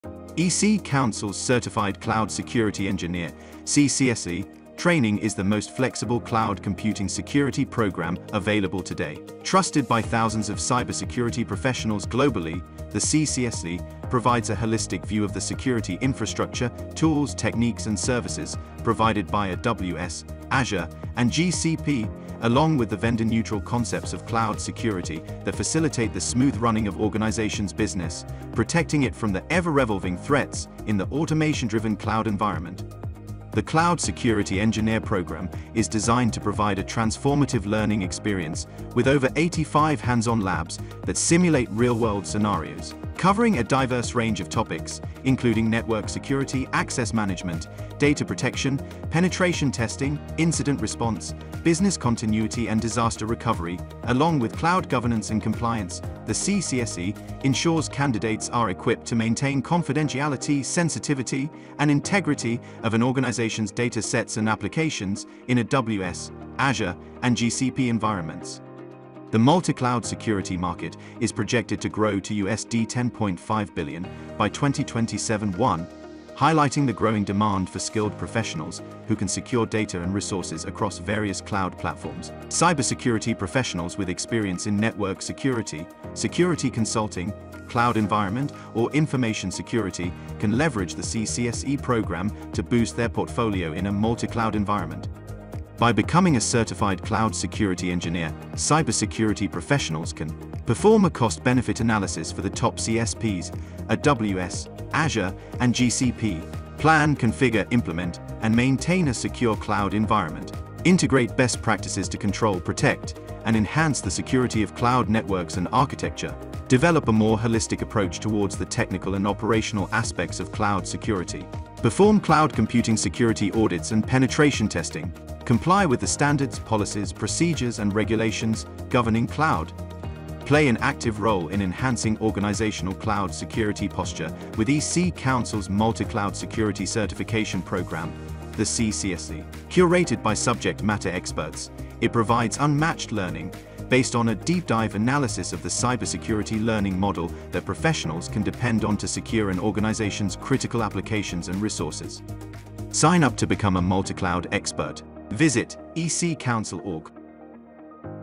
Become a Multi-Cloud Security Expert with EC-Council's C|CSE Certification
With cloud deployments exposing businesses to sophisticated cyber threats, ensuring multi-cloud security is becoming a strategic priority. Candidates wanting to advance their careers in the cloud must be skilled in configuring multi-cloud environments to enable organizations to achieve resilience and improve their security posture.

Gain multi-cloud security skills with EC-Council's Certified Cloud Security Engineer (C|CSE) program — a unique cloud security certification that blends vendor-neutral and vendor-specific cloud security concepts. With hands-on training through 50+ complex labs, the C|CSE cloud security training equips candidates with practical skills to tackle industry-level challenges in the three major CSPs (AWS, Azure, and GCP). A specialized program created in collaboration with subject matter experts from all over the world, the C|CSE cloud certification offers detailed and methodological training, enabling candidates to learn the latest security practices, tools, and techniques and get job-ready for over 20+ cloud security job roles.